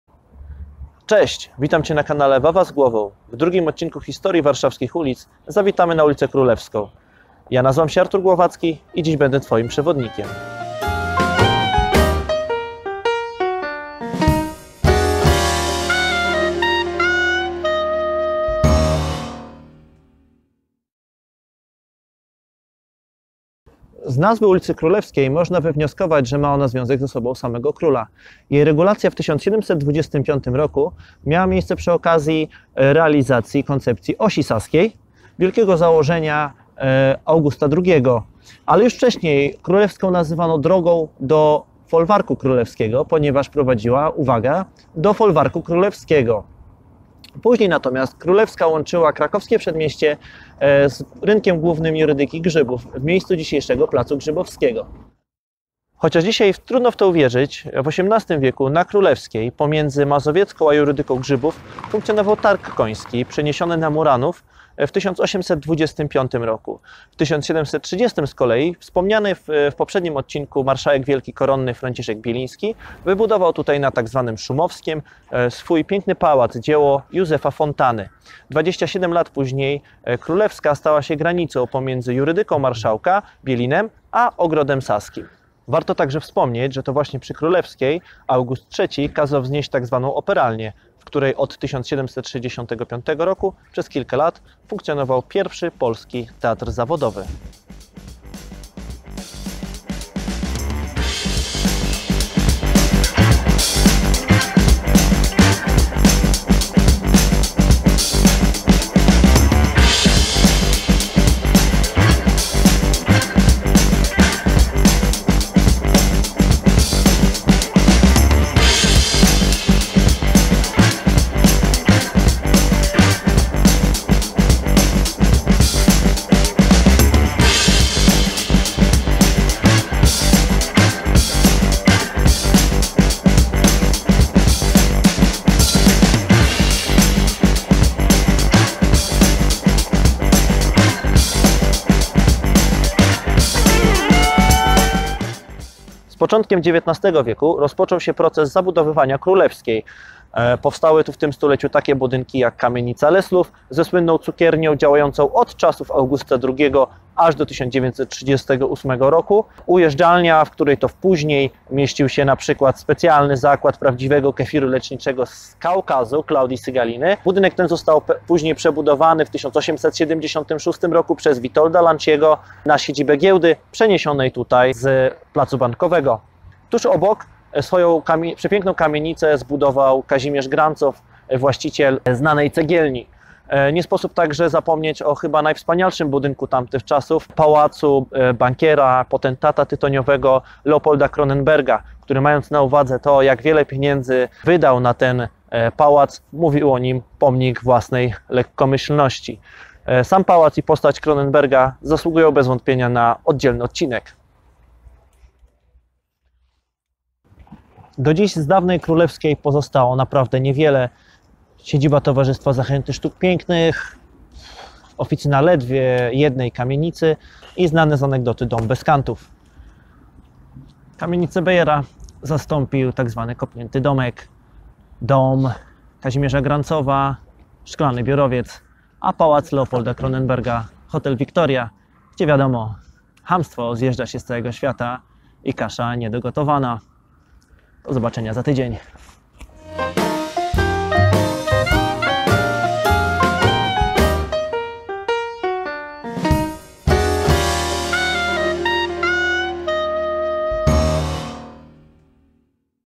Historia warszawskich ulic || odc. 2 || Królewska || Wawa z Głową
Dziś wybieramy się na ulicę Królewską. Skąd wzięła się jej nazwa? Jakie budynki tam kiedyś stały? Czym handlowano w XVIII wieku?

To wszystko w drugim odcinku Historii warszawskich ulic.

Muzyka:
Biblioteka YouTube

Zdjęcia:
Koń :
Designed by Archjoe / Freepik

Domena publiczna:
Pałac Bielińskich, polona.pl
Pałac Kronenberga, polona.pl
Królewska, polona.pl
Budynek Giełdy, polona.pl
August II, Louis de Silvestre, wikipedia.org